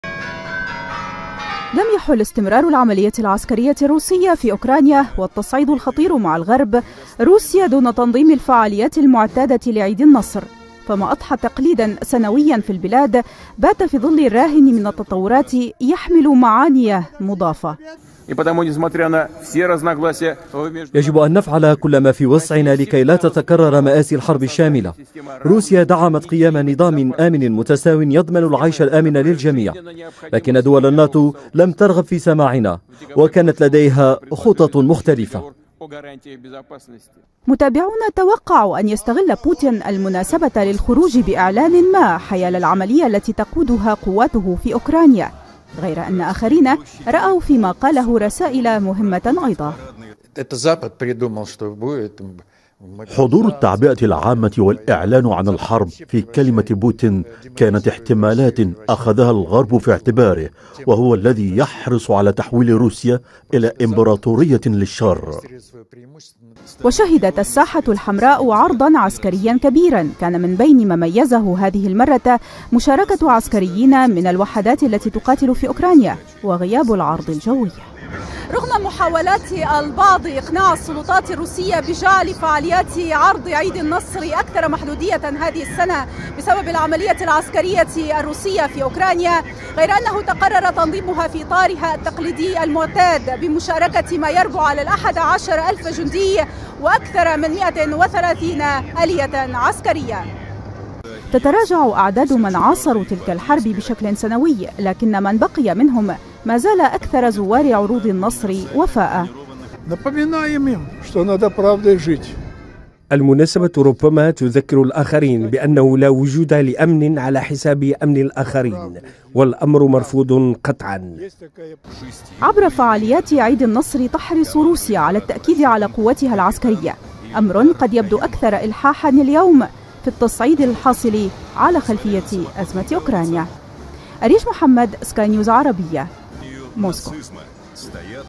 روسيا تحيي الذكرى الـ 77 للانتصار على النازية في الحرب العالمية الثانية
أحيت روسيا الذكرى السابعة والسبعين لانتصار الاتحاد السوفييتي والحلفاء على ألمانيا النازية. وخلال مراسم الاحتفال في الساحة الحمراء بموسكو، أكد الرئيس الروسي فلاديمير بوتن، أن الجيش الروسي يقاتل في أوكرانيا من أجل أمن وطنه، لافتا إلى أن روسيا اضطرت لصدّ عدوان محتوم عليها بتحرك استباقي.